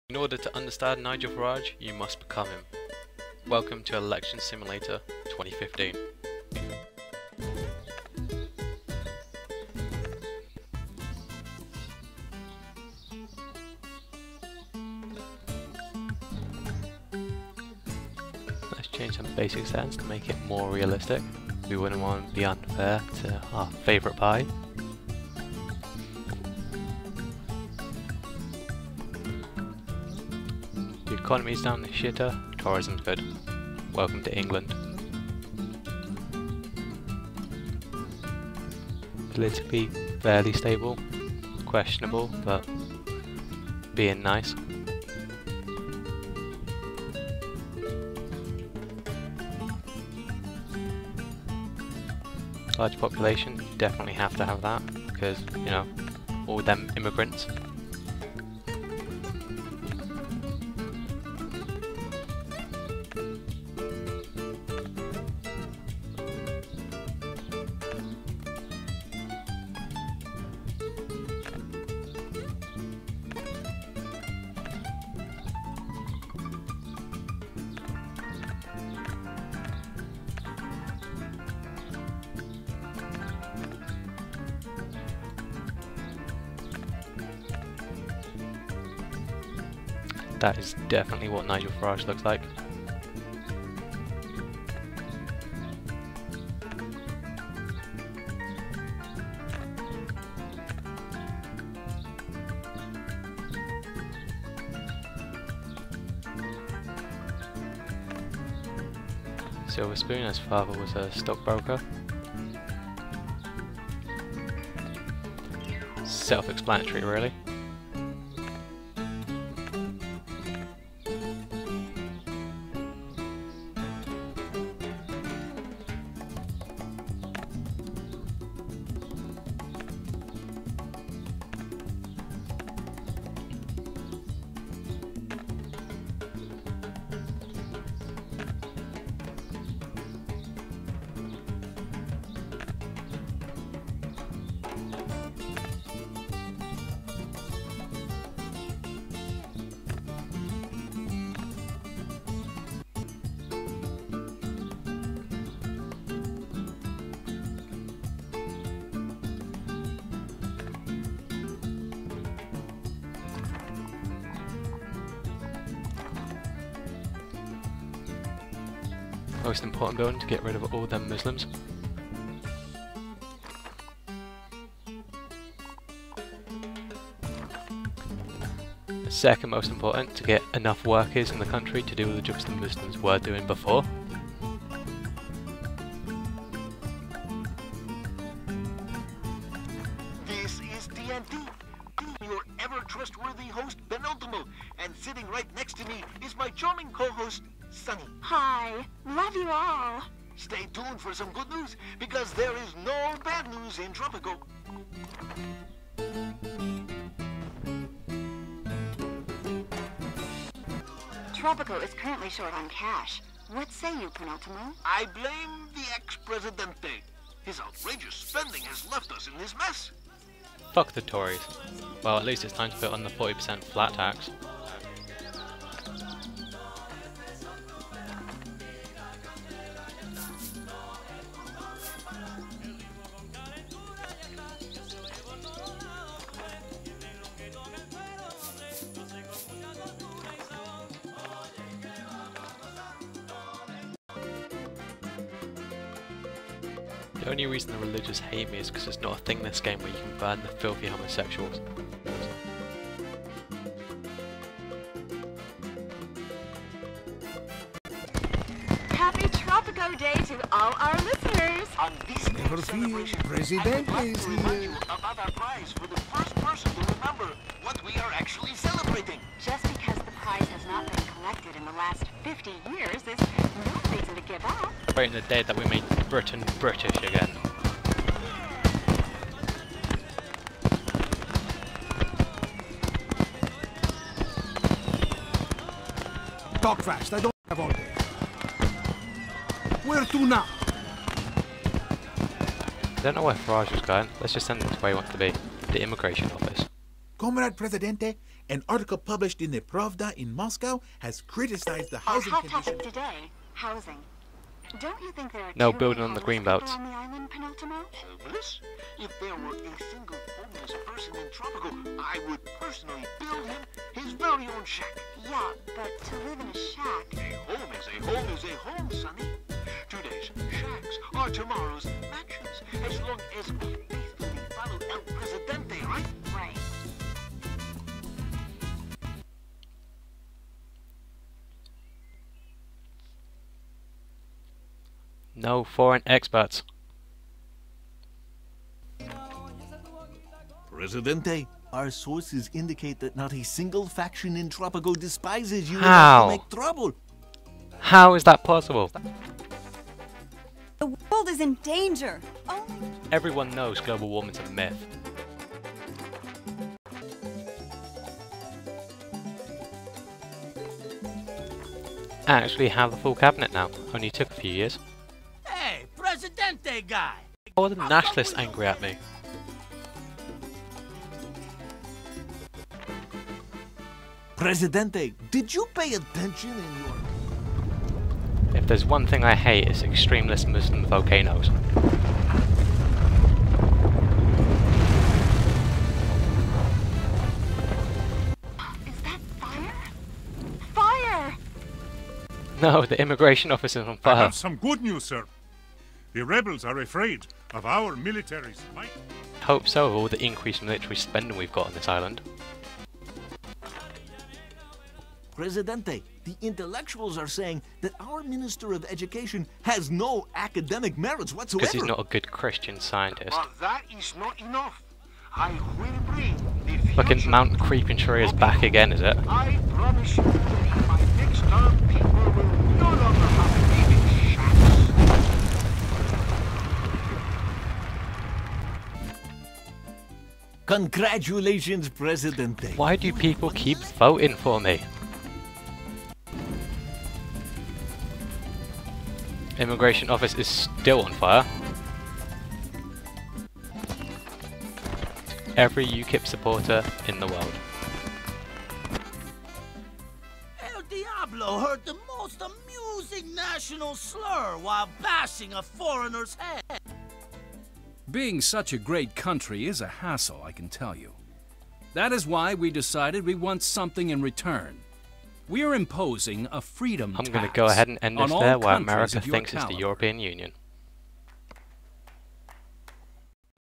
Farages Britain
The year is 2015 and ukip has won the election. only Nigel Farage can save Britain and make it great once again. watch as all becomes right in the world though the dedicated ukip supporters.